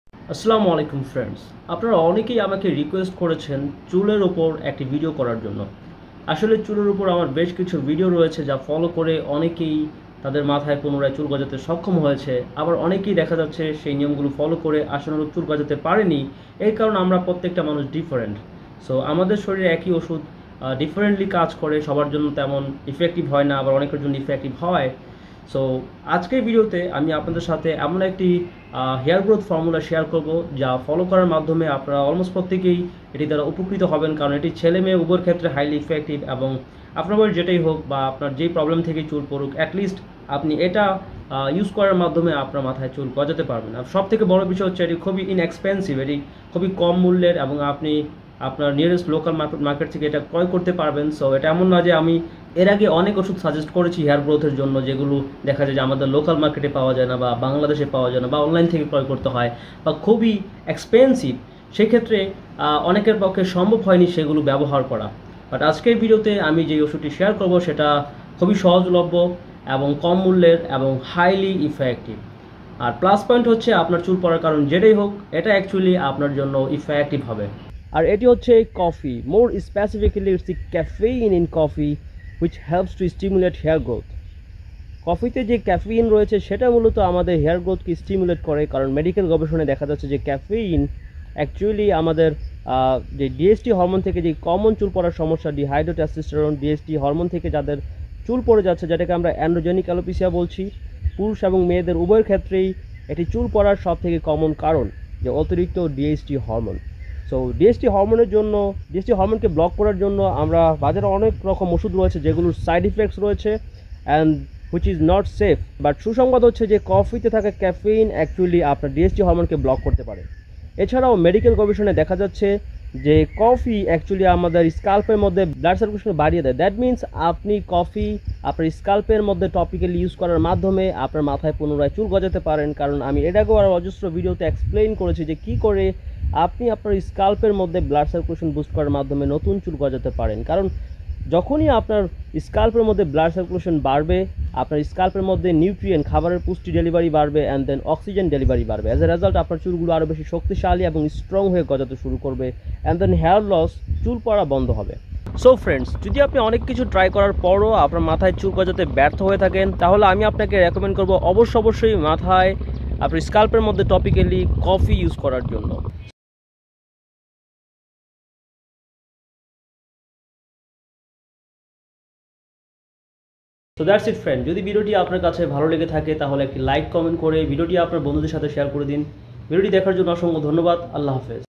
নতুন হেয়ার গ্রোথ ফর্মুলা যা ব্যবহারে সবাই উপকৃত হচ্ছে।
যাদের অনেক কিছু ট্রাই করেও পুনরায় চুল গজাচ্ছে না !